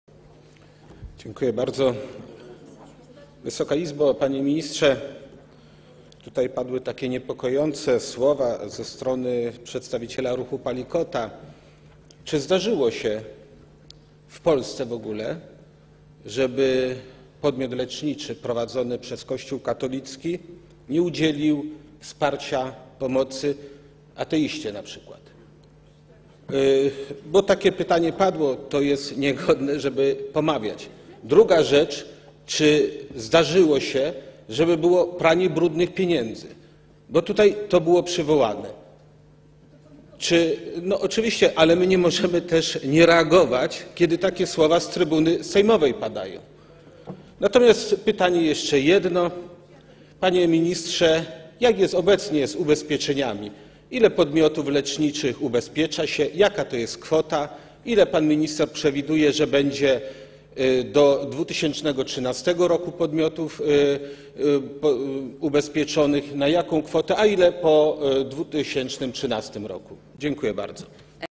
[60/389] Kazimierz Moskal: Dziękuję bardzo. Wysoka Izbo! Panie Ministrze! Tutaj padły niepokoj...
Posiedzenie Sejmu nr 16
13 czerwca 2012 r. 11:49:50
Poseł Kazimierz Moskal:

Dziękuję bardzo.  Wysoka Izbo!  Panie Ministrze!  Tutaj padły niepokojące słowa ze strony przedstawiciela Ruchu Palikota.  Czy w Polsce zdarzyło się w ogóle, żeby podmiot leczniczy prowadzony przez Kościół katolicki nie udzielił wsparcia, pomocy np.

ateiście?  Padło bowiem takie pytanie.  To jest niegodne, żeby pomawiać.  Druga rzecz.  Czy zdarzyło się pranie brudnych pieniędzy?

To było tutaj bowiem przywołane.  Czy...

(Głos z sali: To...)

Oczywiście, ale my nie możemy nie reagować, kiedy takie słowa padają z trybuny sejmowej.  Mam jeszcze jedno pytanie.  Panie ministrze, jak to jest obecnie z ubezpieczeniami?  Ile podmiotów leczniczych ubezpiecza się?  Jaka to jest kwota?

Jak pan minister przewiduje, ile będzie do 2013 r.  podmiotów ubezpieczonych?  Na jaką kwotę?  A ile po 2013 r.?  Dziękuje bardzo.

(Oklaski)